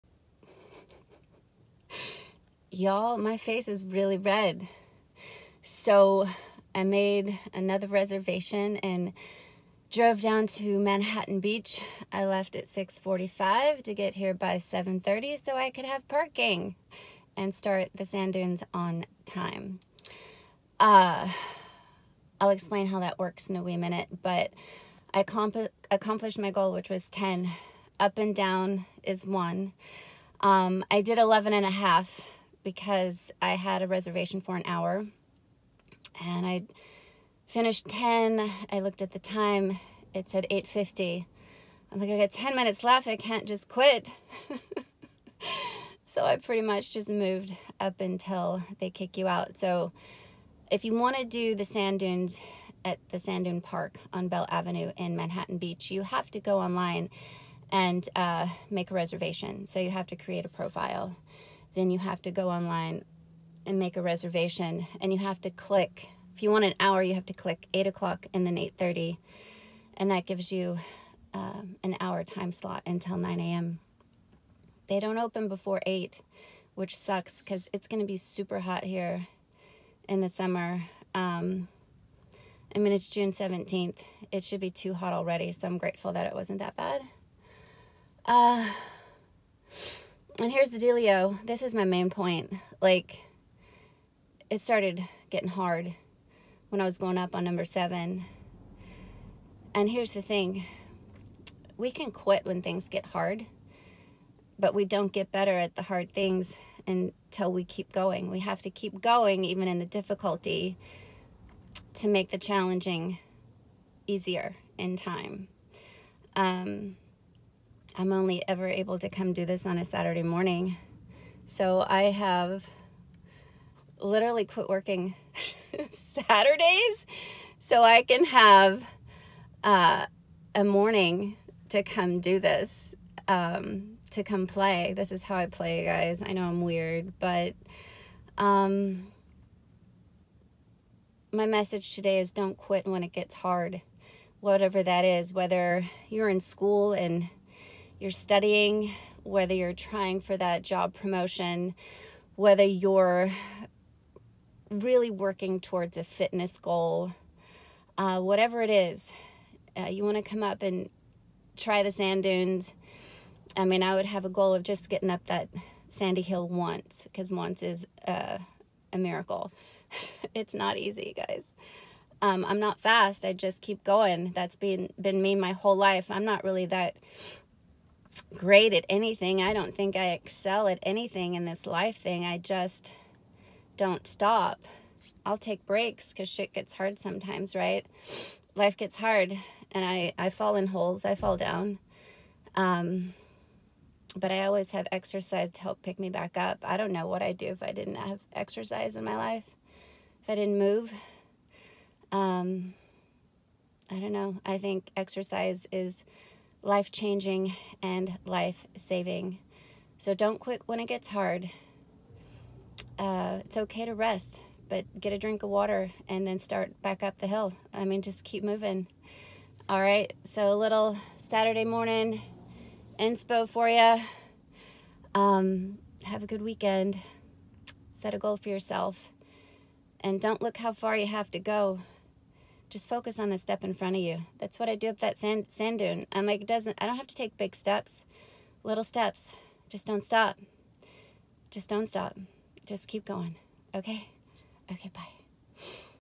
Just Don’t Stop.
A little inspiration after completing my workout at Sand Dune Park in Manhattan Beach.